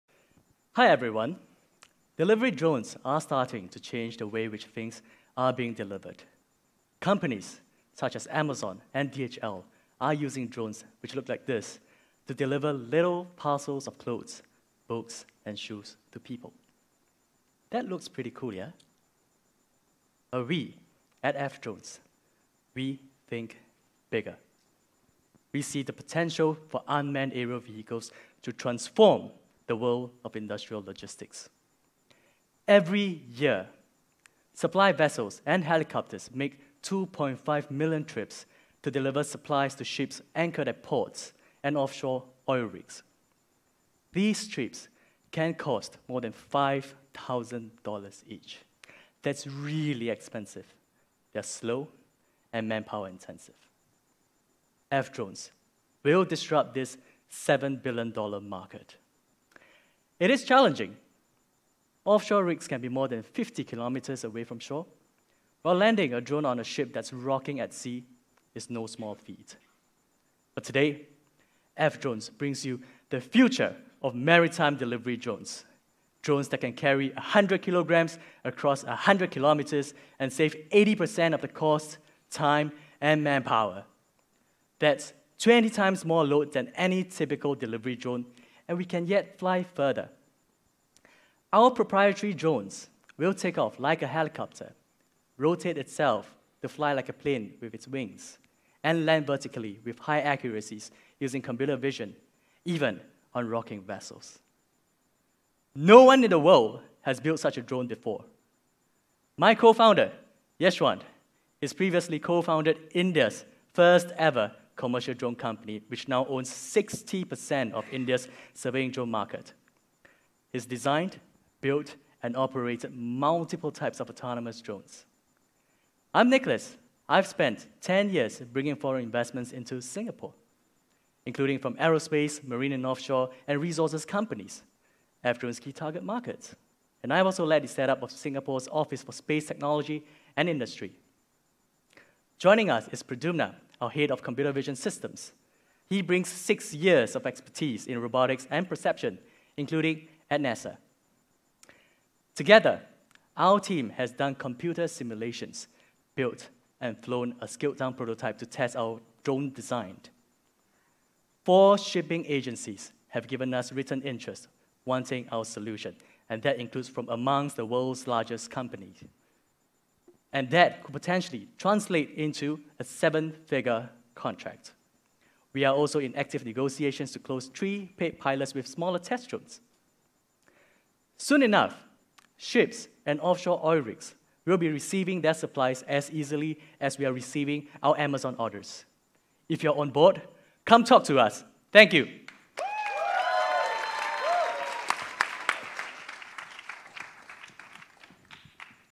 F-drones @ EF Investor Day
Learn more about F-drones and how its innovative solution enables large-scale and on-demand aerial transportation of goods for the maritime industry!